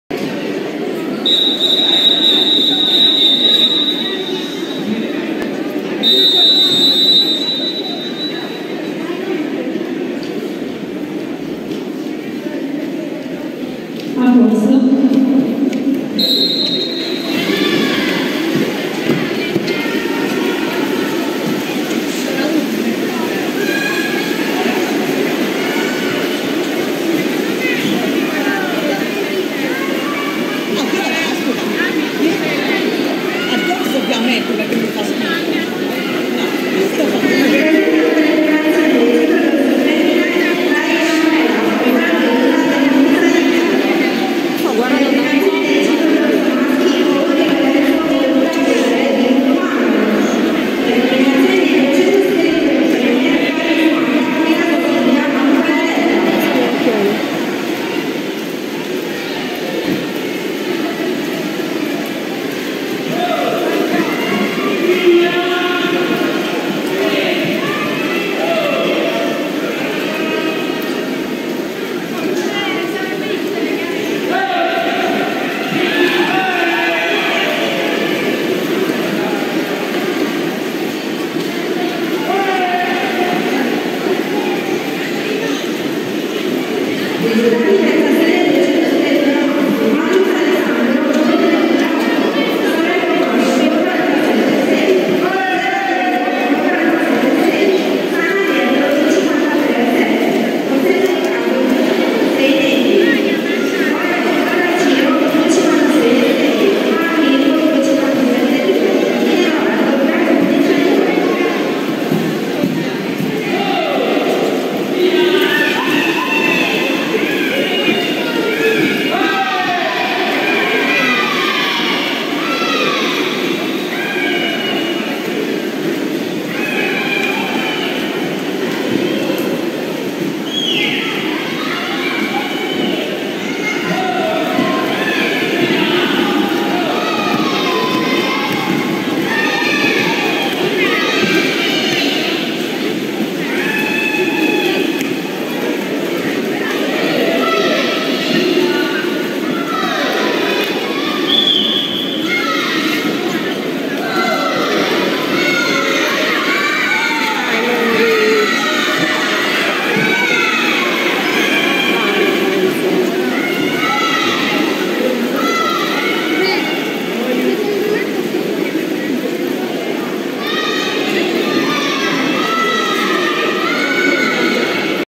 Finali Regionali Nuoto Esordienti A marzo 2022 - 200 Libero Pitzanti Lorenzo
Nuoto Sardegna Finali Campionati Regionali Nuoto Vasca Corta Categoria Esordienti A - ATLANTIDE CAMPIONE REGIONALE marzo 2022 - Gara 200 Libero Pitzanti Lorenzo